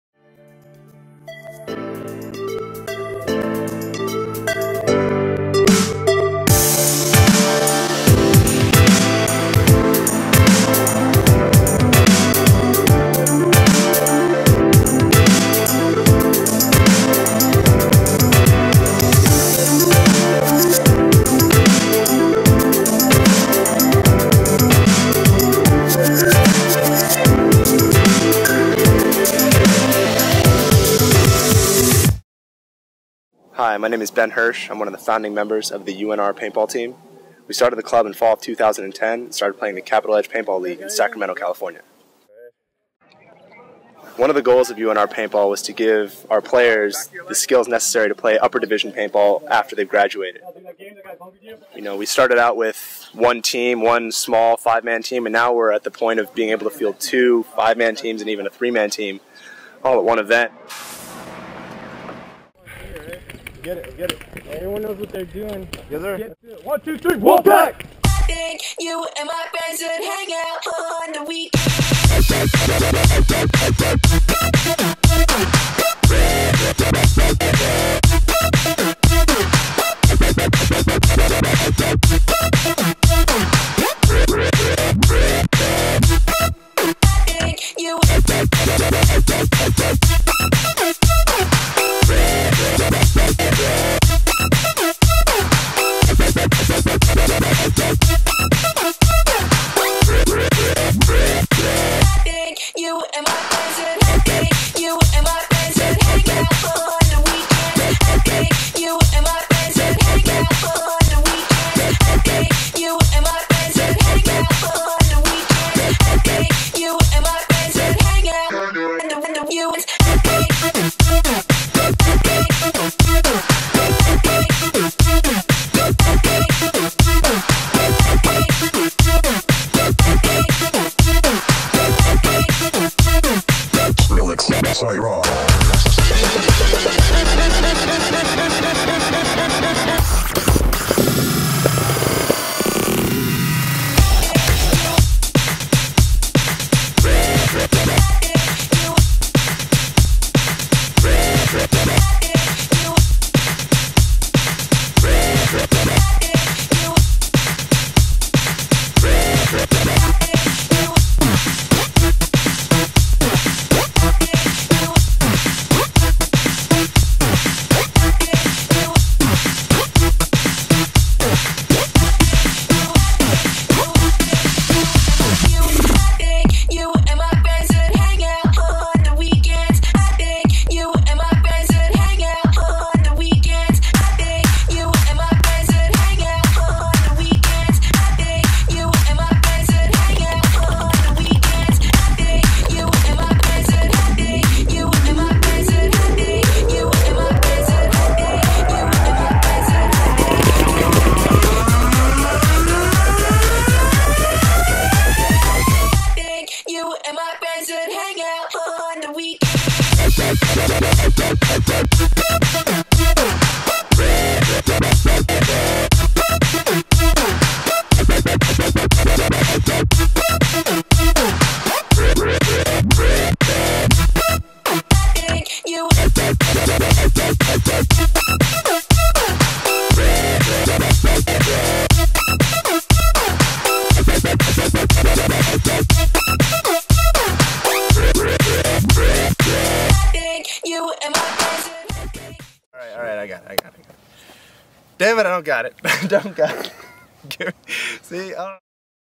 UNR Paintball 2012
Edit for my other paintball team, the University of Nevada. Music credit at the end of the video.
Go Wolf Pack!!